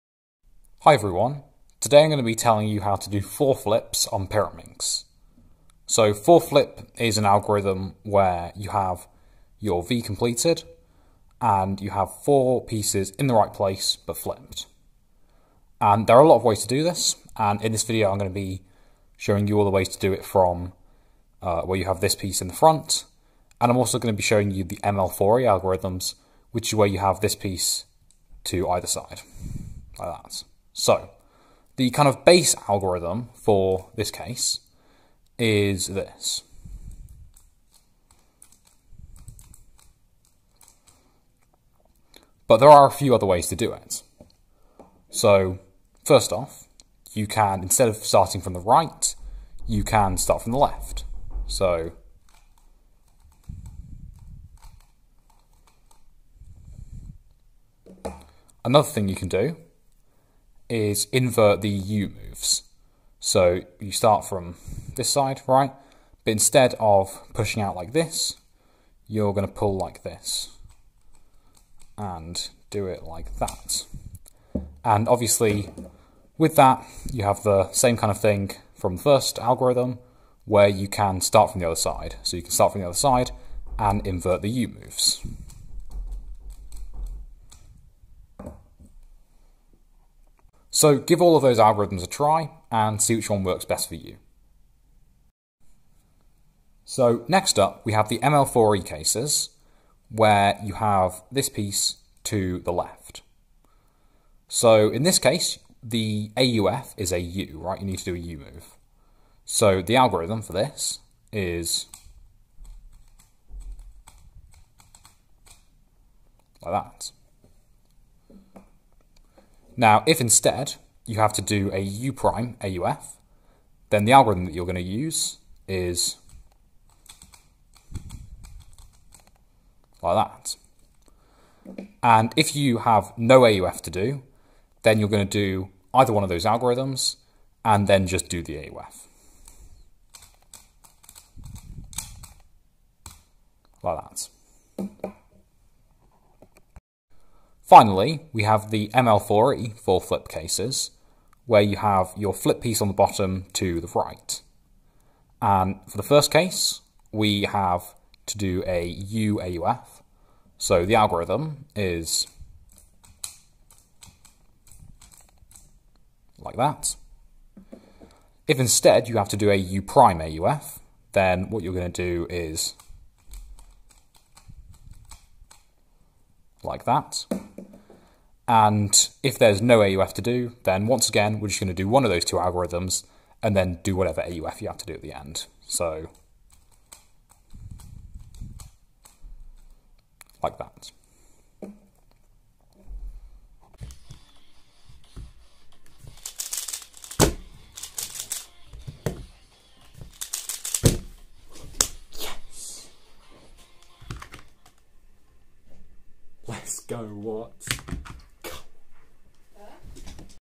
Pyraminx 4-Flip algorithms (including ML4E)
A video showing all the 4-flip cases for Pyraminx.